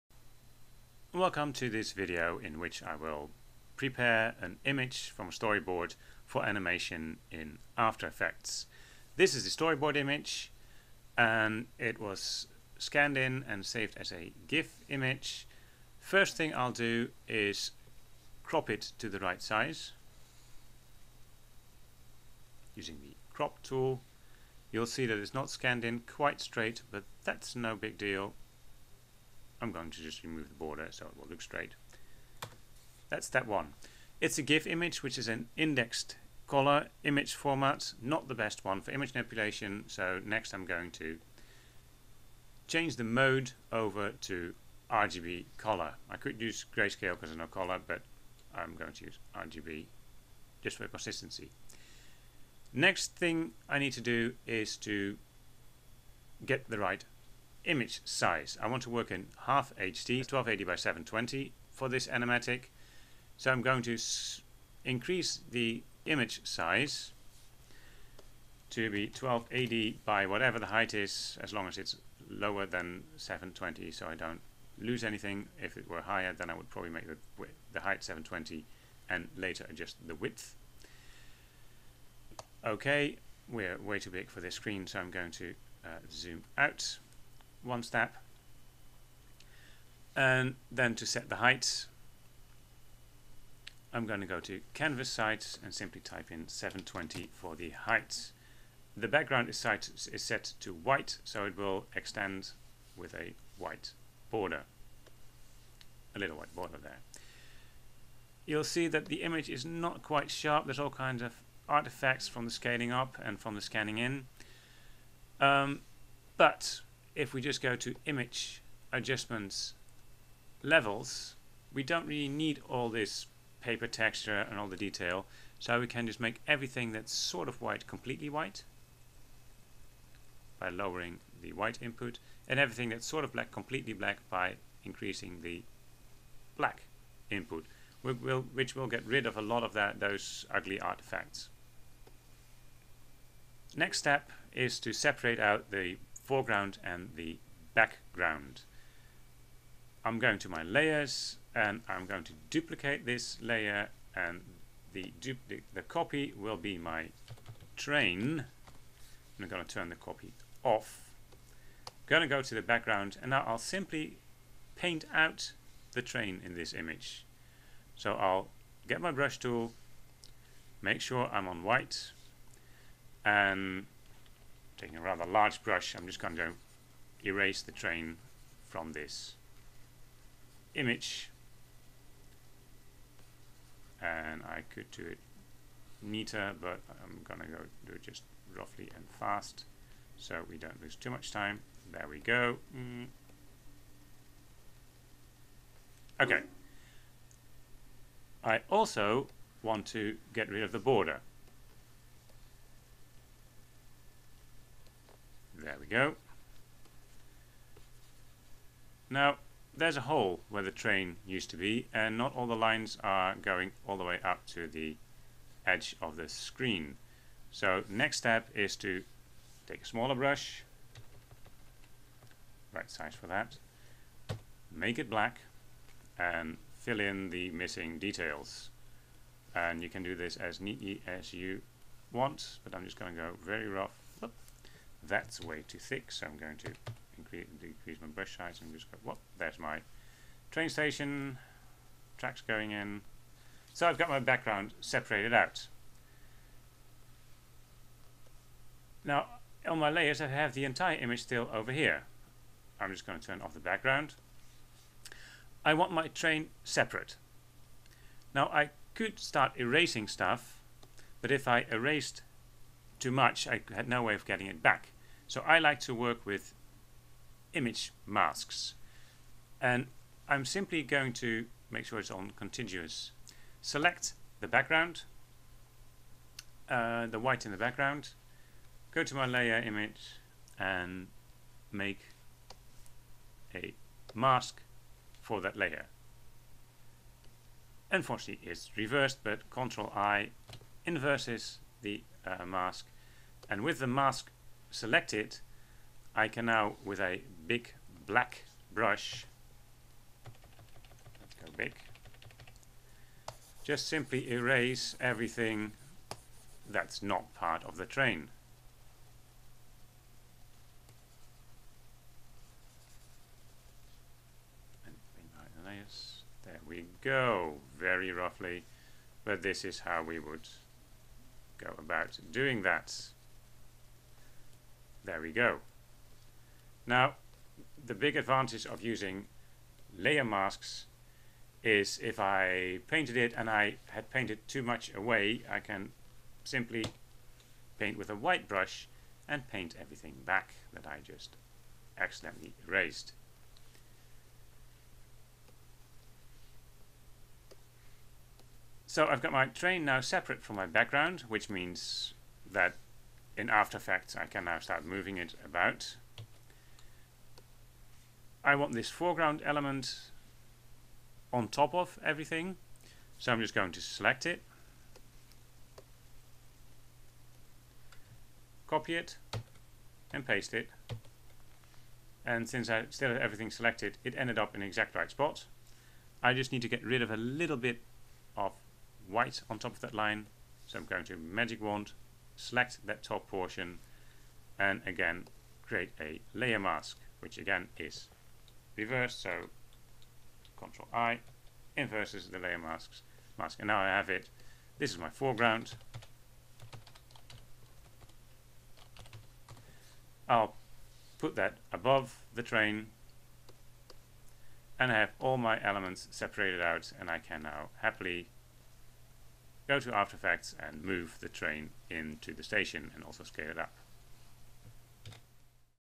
Preparing a Shot for an Animatic in Photoshop
Taking a scanned storyboard image and preparing it to be imported in After Effects so it can be animated to be part of an animatic. How to / show and tell video created for my Animation Production class at Montclair State University. The example is taken from the MetroCAF 2009 Opening animation the artwork is by Anna Kania (now Leung)